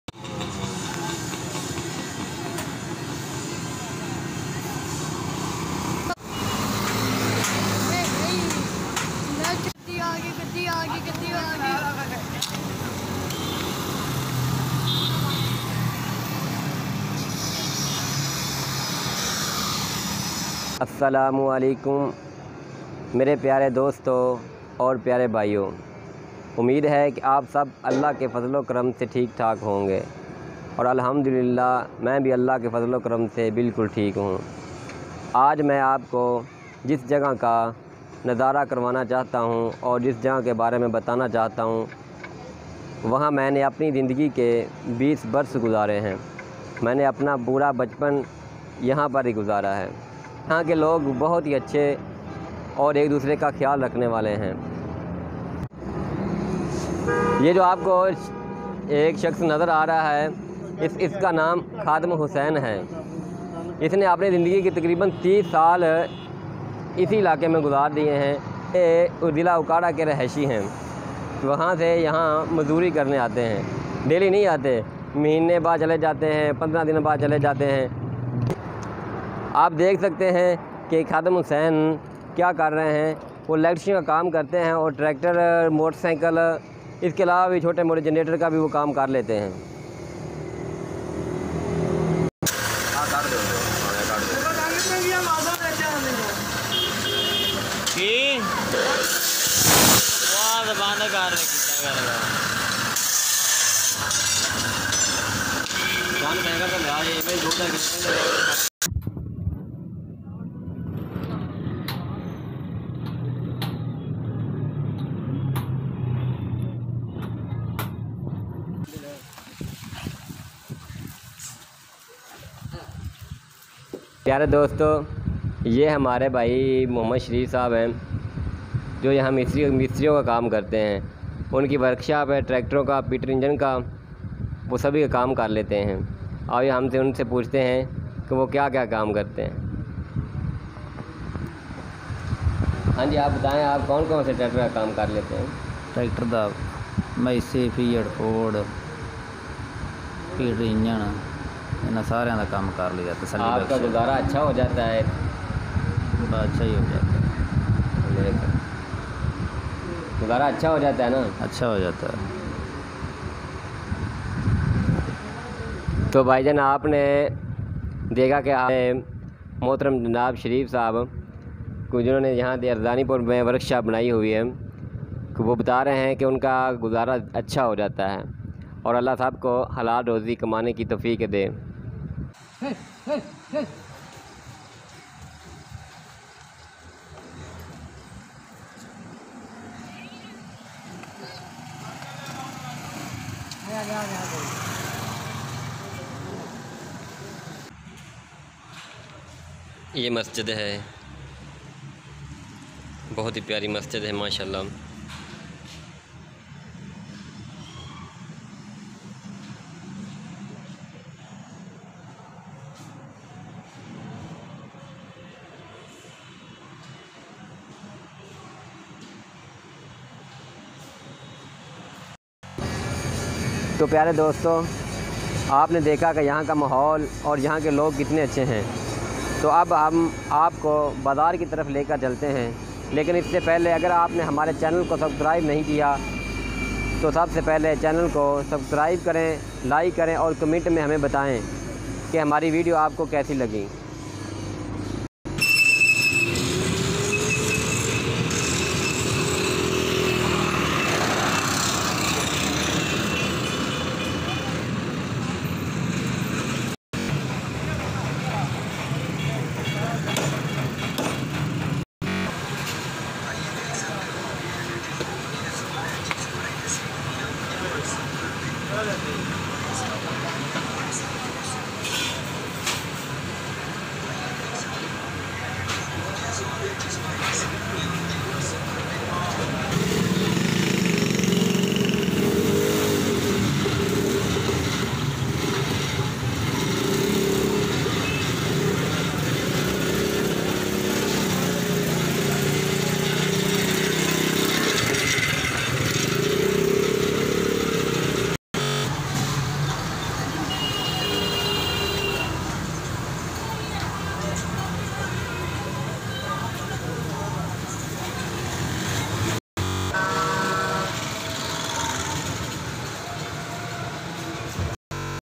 Village Arzani pur ارزانی پور | naeem pakistani
چینل کو ضرور سبسکرائب کریں
This place very beautiful and his people is also beautiful.in this place ,i am living a long time.